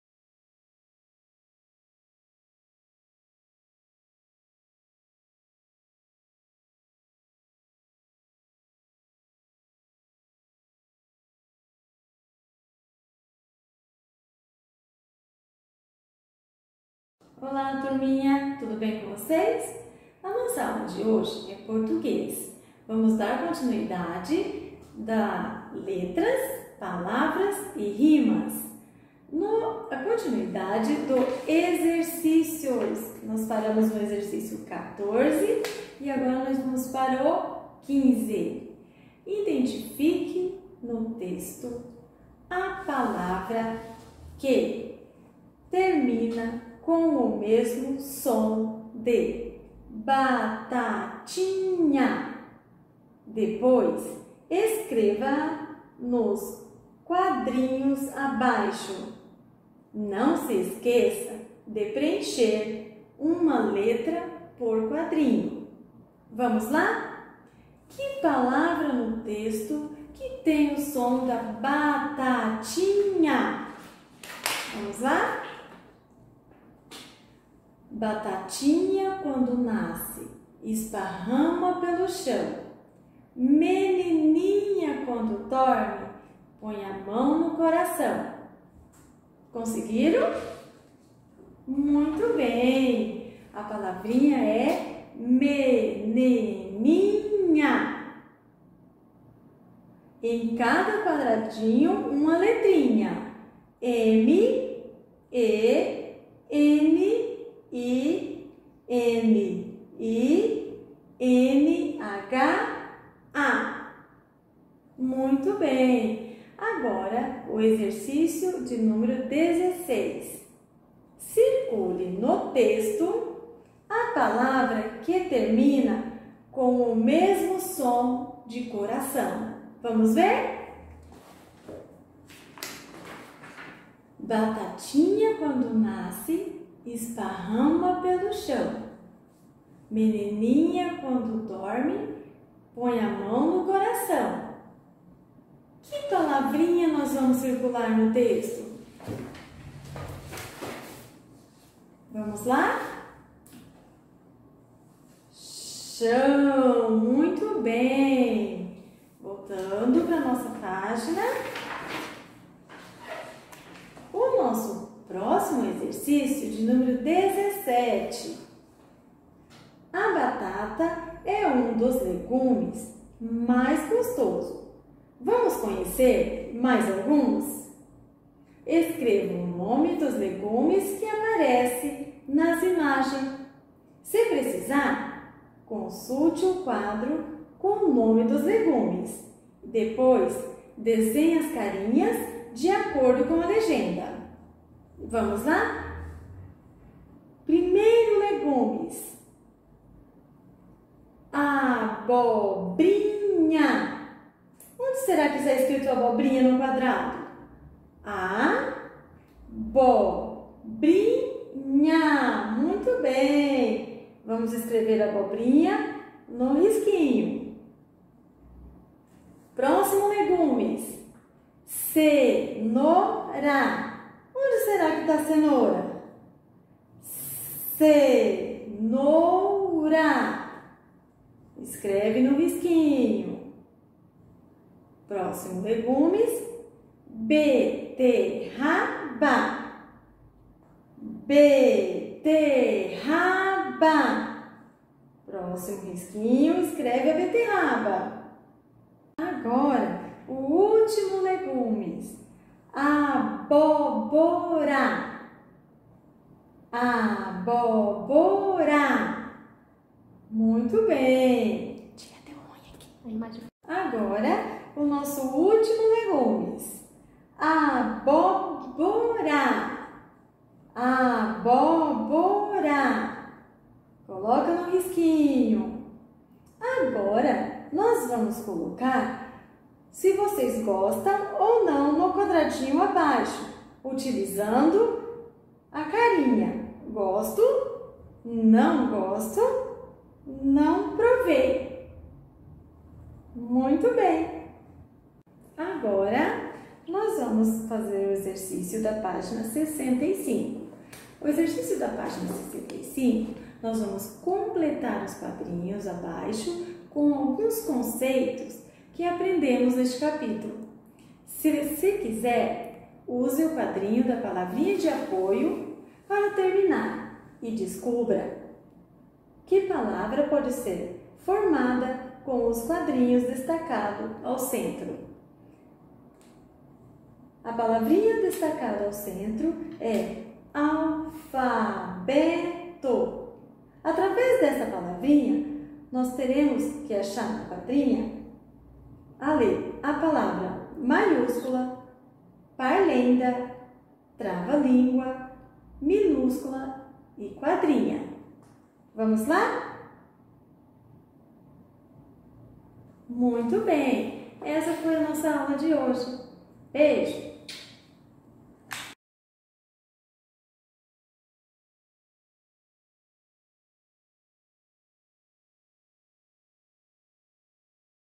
Vídeo Aula da Professora Ligislene - Português
Componente curricular: Português
Conteúdo: Letras, Palavras e Rimas. Continuação dos exercícios.